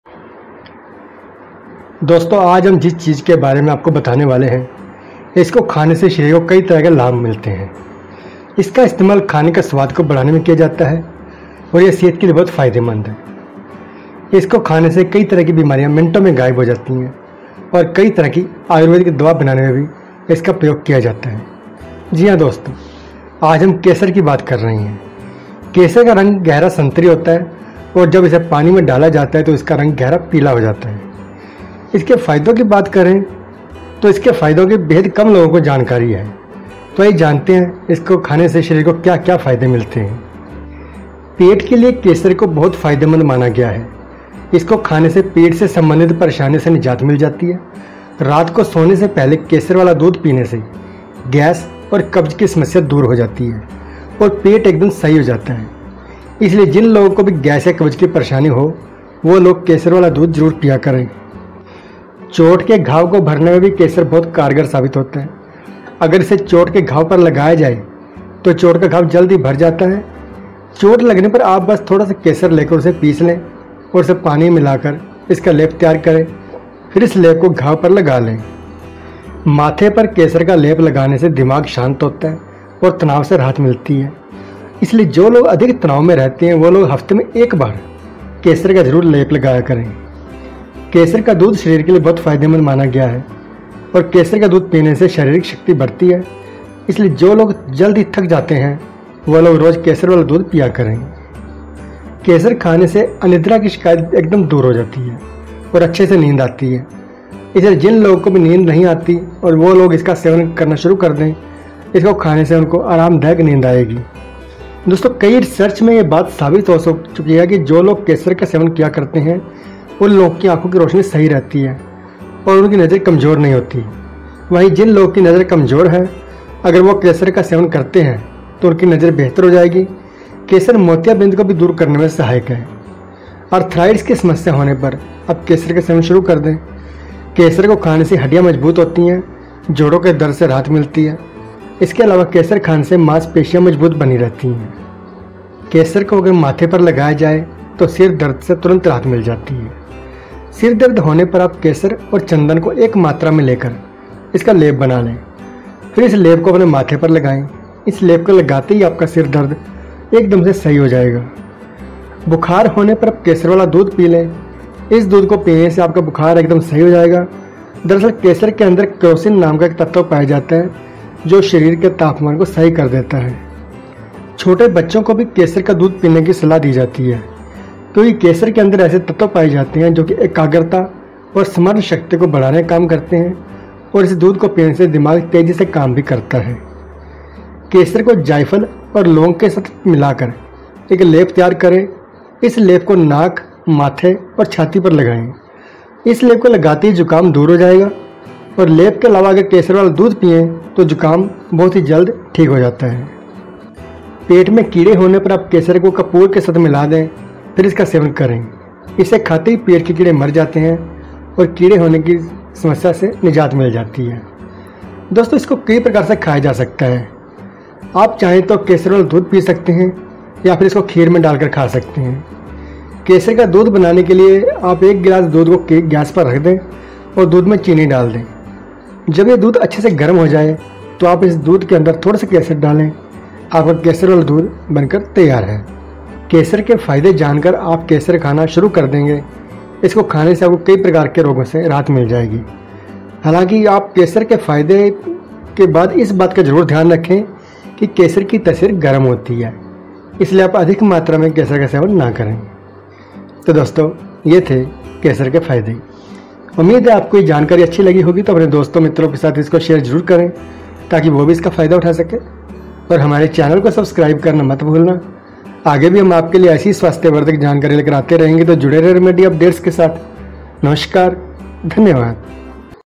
रोजाना दुध में डालकर खाना शुरु करे, कमजोरी,अर्थराईटस,आंखों की रोशनी, सिरदर्द, जैसी परेशानी नही होगी
रोजाना दुध में डालकर खाना शुरु करे, कमजोरी,अर्थराईटस,आंखों की रोशनी, सिरदर्द,बुखार जैसी परेशानी नही होगी